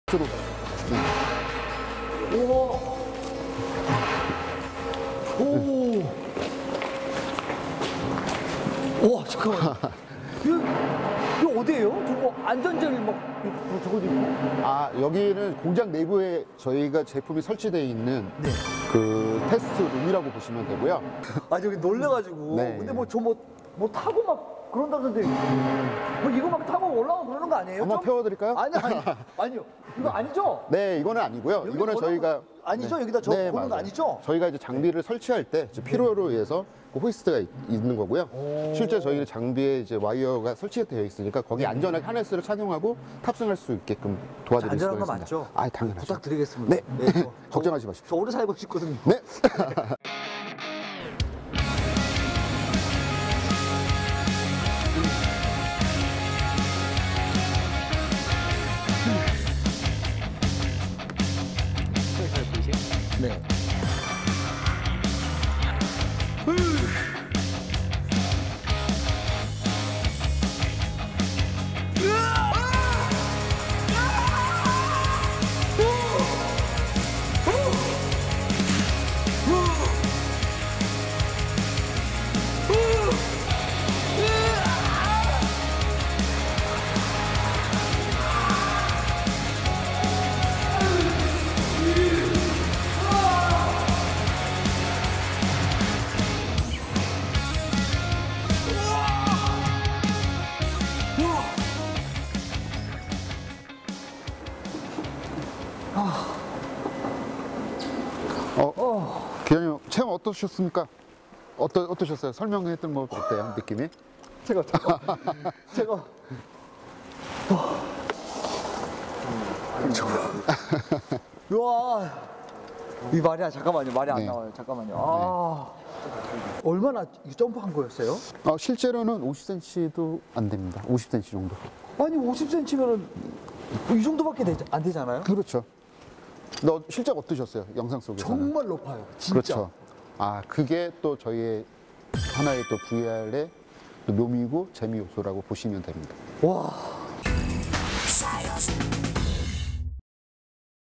SF 영화 속 주인공이 되는 VR 어트랙션 / YTN 사이언스
영화 속 주인공처럼 하늘을 날면서 총을 쏜다면 어떤 기분일까?
바로 이를 가능하게 해주는 것이 VR 어트랙션이라고 한다.
출시되기 전 테스트를 거치게 되는데, 함께 체험해본다.